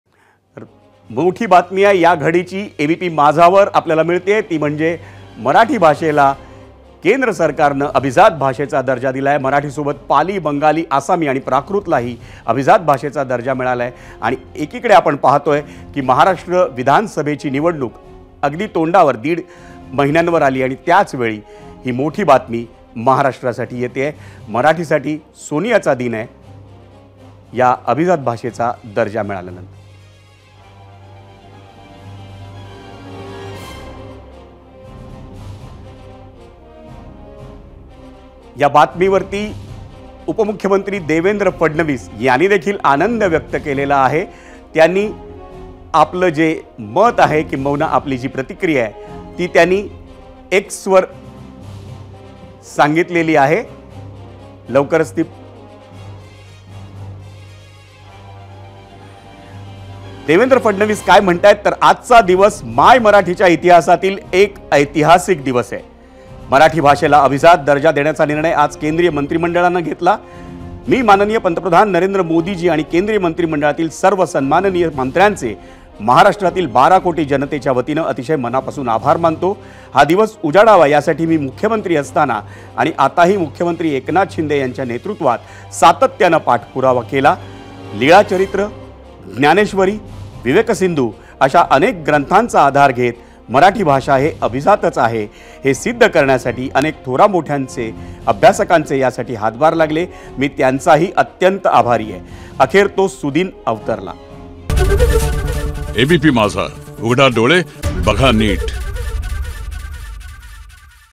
Marathi Bhasa Abhijat Darja | विधानसभेंच्या तोडांवर मोठा निर्णय, मराठीला अभिजात दर्जा
ABP Majha Live TV | Navaratri 2024 | Vidhan Sabha Election | Thackeray vs Shinde | Nilesh Rane